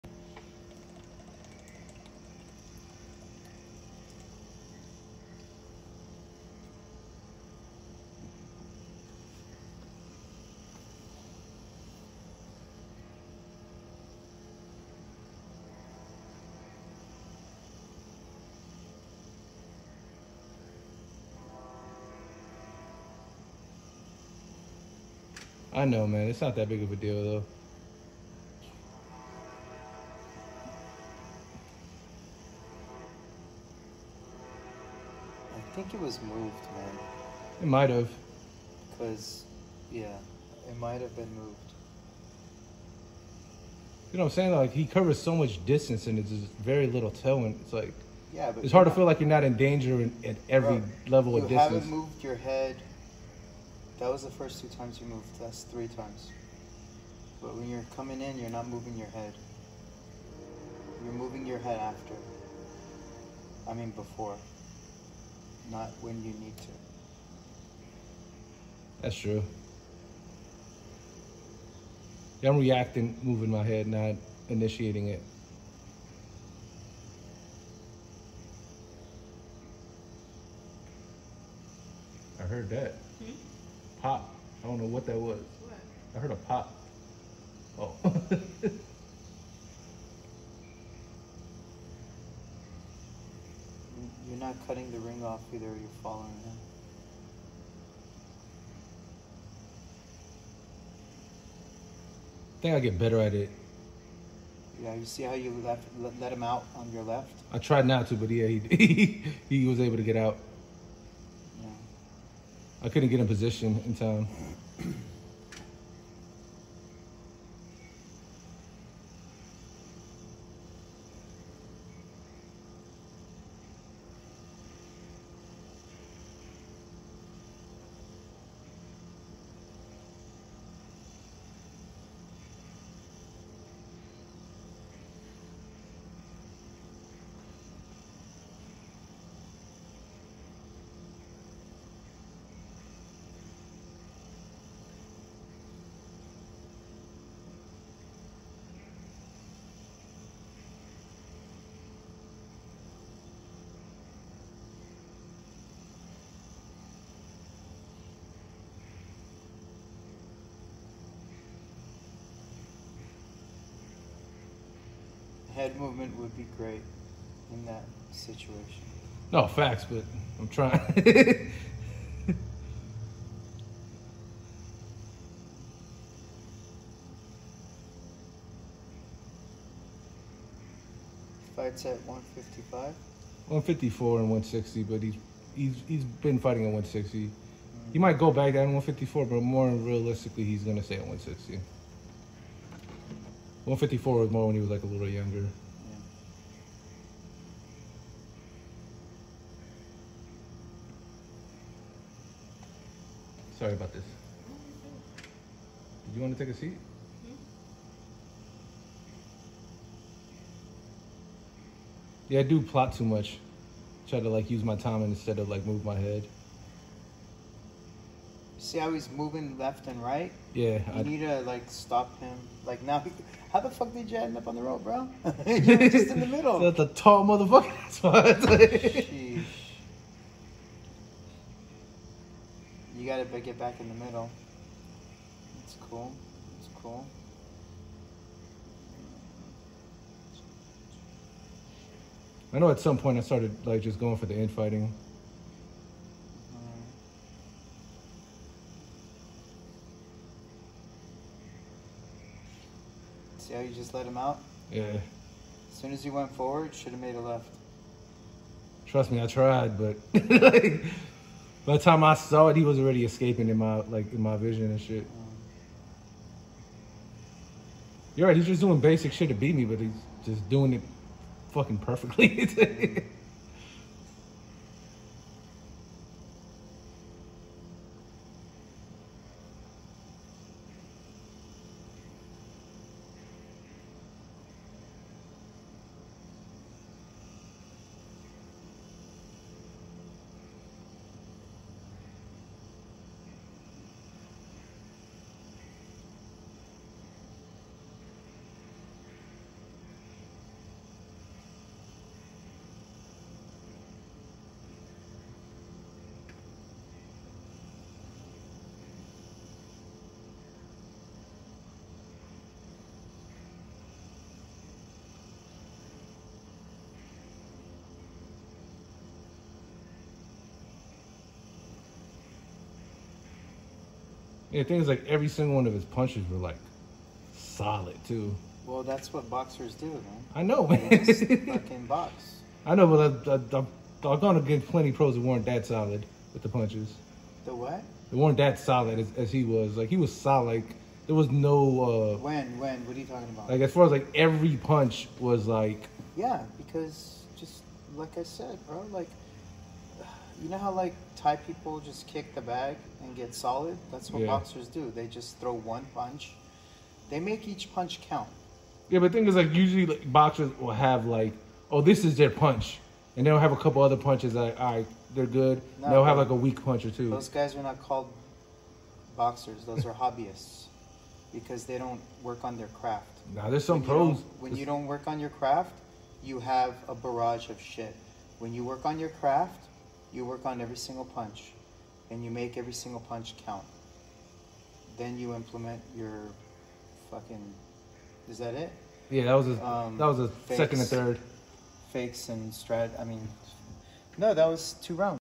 Footage of me sparring my friends a undefeated pro boxer
Me sparring my homies who is a undefeated pro boxer.The footage is recorded off my phone from a PC the recorded the footage for a week or two before deleting automatically.This is round 2-3 of 3.